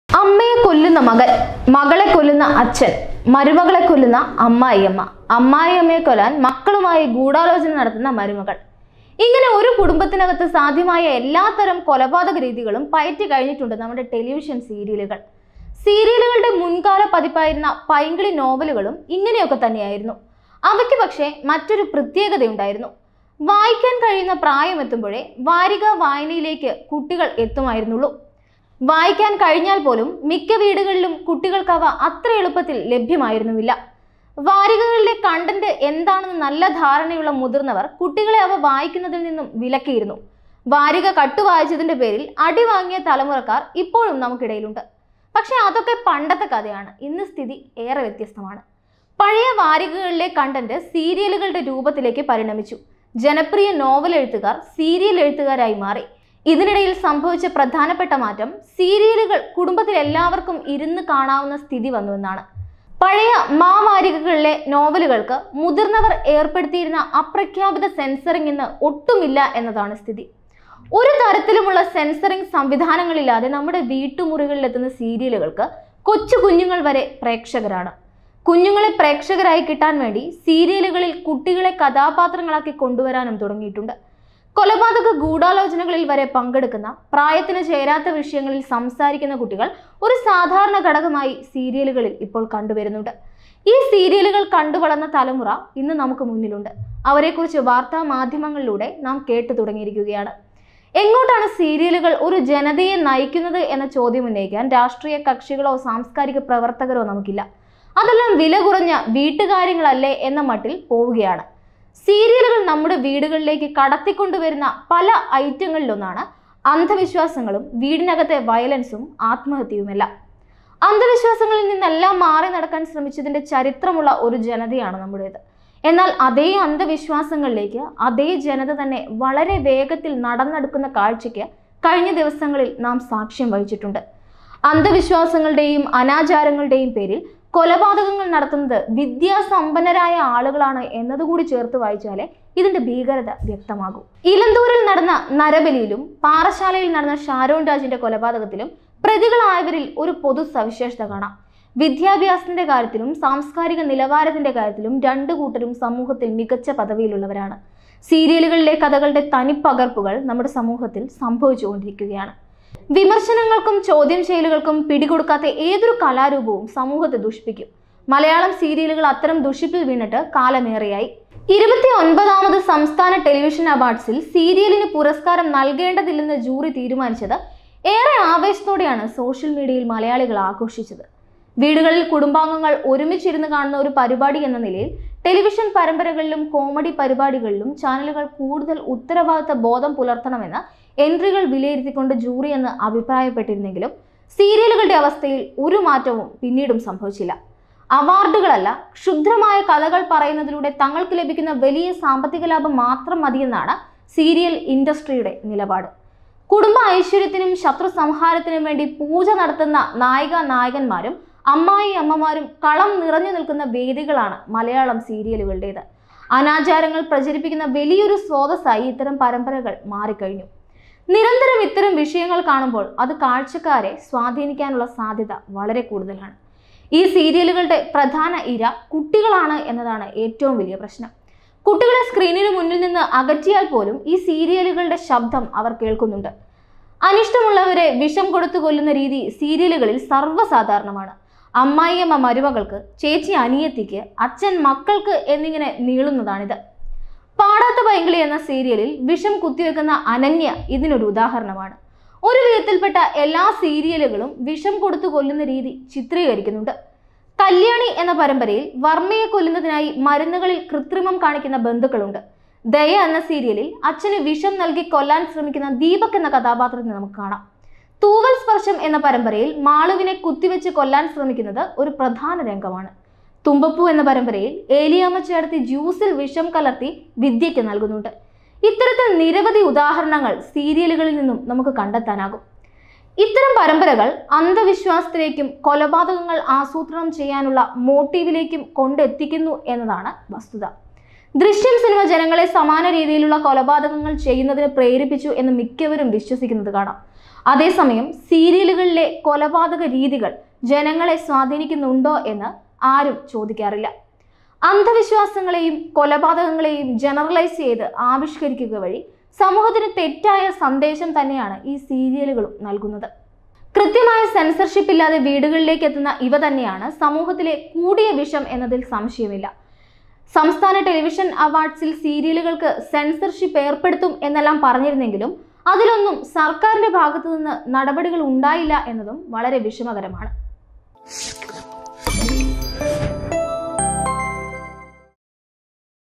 സീരിയലുകളിൽ നിന്ന് നാം പഠിക്കുന്നതെന്ത്? | Malayalam serial | Serials
അമ്മയെ കൊല്ലുന്ന മകൻ, മകളെ കൊല്ലുന്ന അച്ഛൻ, മരുമകളെ കൊല്ലുന്ന അമ്മായിയമ്മ, അമ്മായിയമ്മയെ കൊല്ലാൻ മക്കളുമായി ഗൂഢാലോചന ചെയ്യുന്ന മരുമകൾ എന്നിങ്ങനെ ഒരു കുടുംബത്തിനകത്ത് സാധ്യമായ എല്ലാത്തരം കൊലപാതക രീതികളും പയറ്റിക്കഴിഞ്ഞിട്ടുണ്ട് നമ്മുടെ ടെലിവിഷൻ സീരിയലുകൾ.